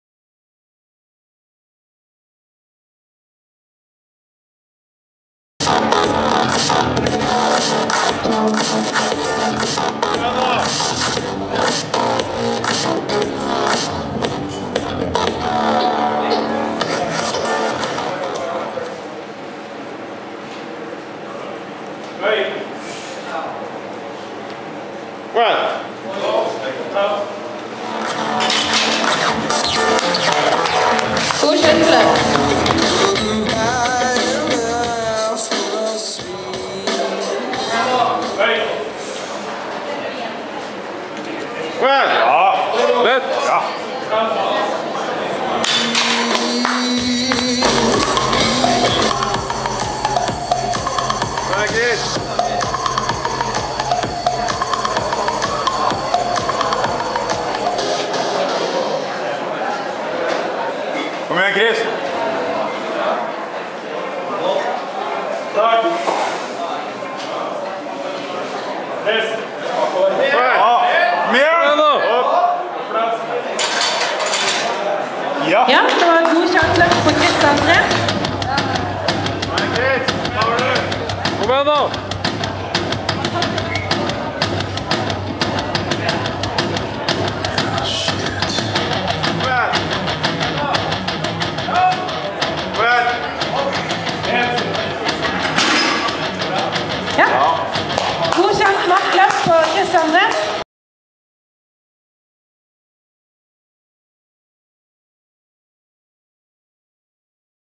Chris Ruud, nasjonalt utstyrsfritt 2014 (raw). Klasse u -120 kg, debutant
Chris er 17 år, var debutant og løftet godt over treningspersene, kom seg igjennom med 8 av 9 godkjente løft.
Det ble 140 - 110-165 = 415 kg.
Det ga en andre plass i klassen.
Vi gleder oss til fortsetelsen.